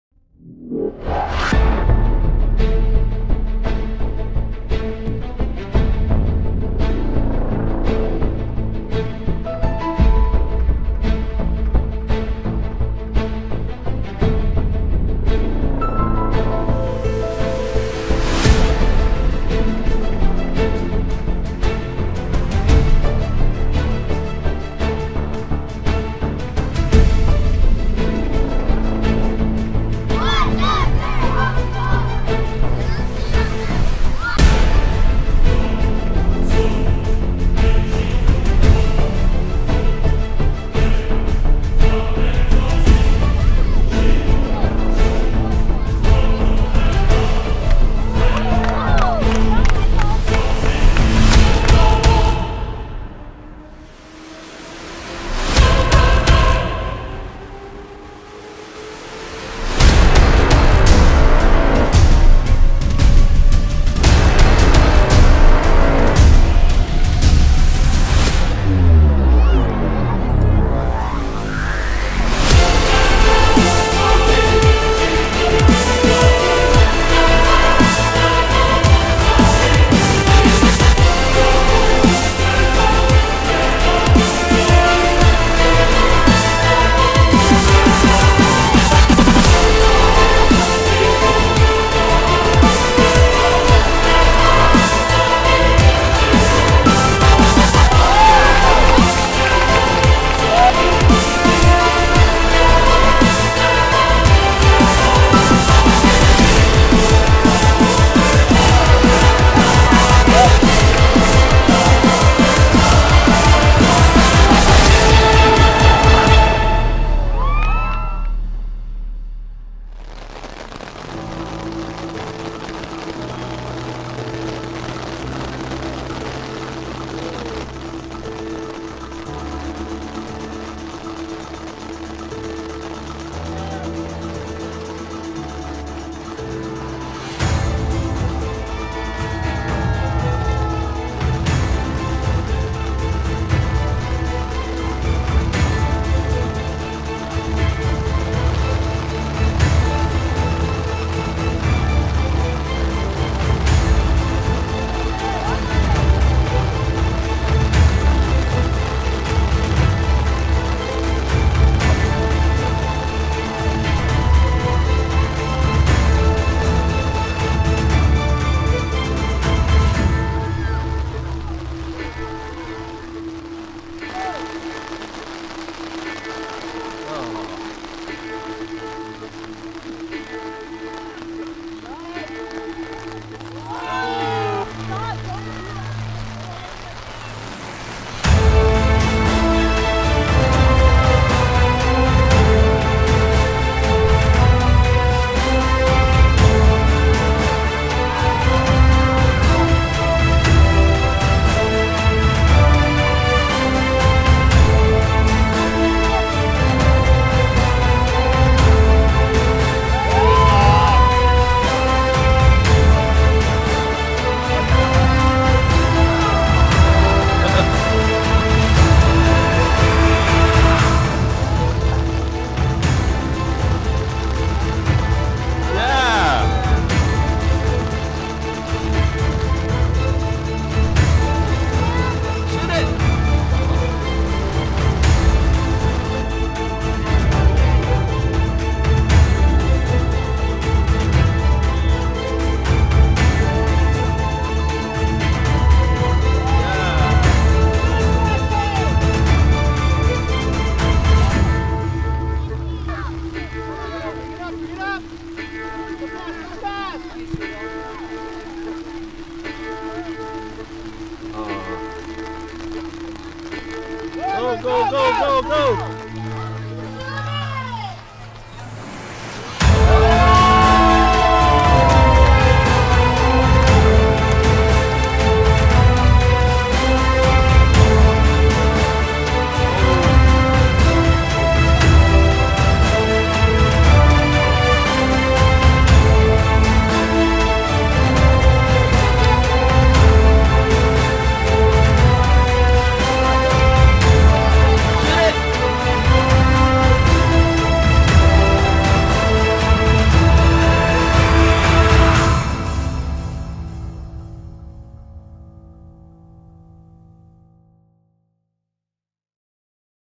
Hokuala Boys 07 08 Sept 10 2017
The Soccer Season opened this weekend!

Game one was a very exciting game with the 07 Premiere Hokuala Boys vs Galaxy which ended up a tied game 3-3

Game two was the 08 Hokuala Boys vs Crush. The battled through some heavy down pour of rain almost the entire game. The 08 boys came out with a very exciting win for their first game of the season.

Video was shot with DJI Osmo+ mounted to a tripod.